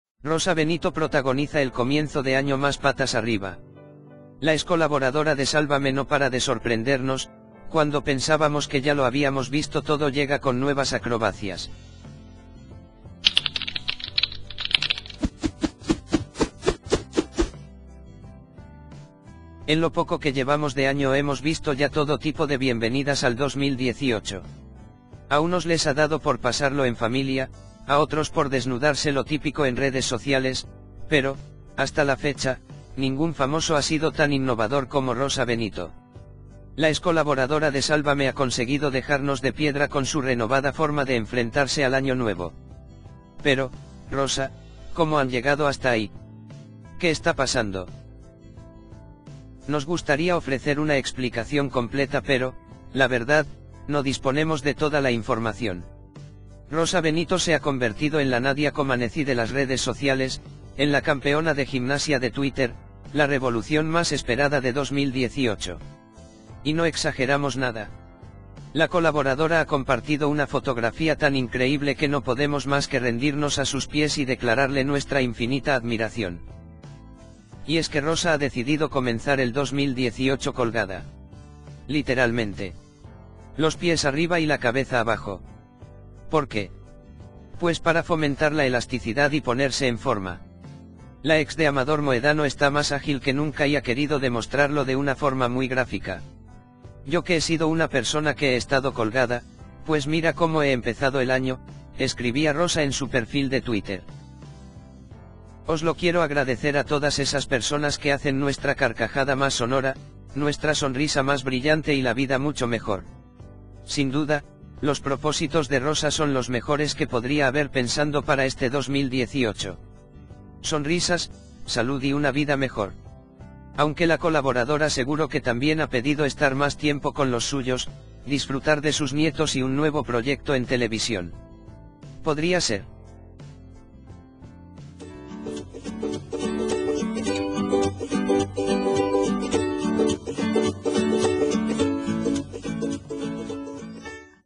Rosa Benito protagoniza el comienzo de año más 'patas arriba'
★ Resumen ➜ La excolaboradora de 'Sálvame' no para de sorprendernos; cuando pensábamos que ya lo habíamos visto todo llega con nuevas acrobacias...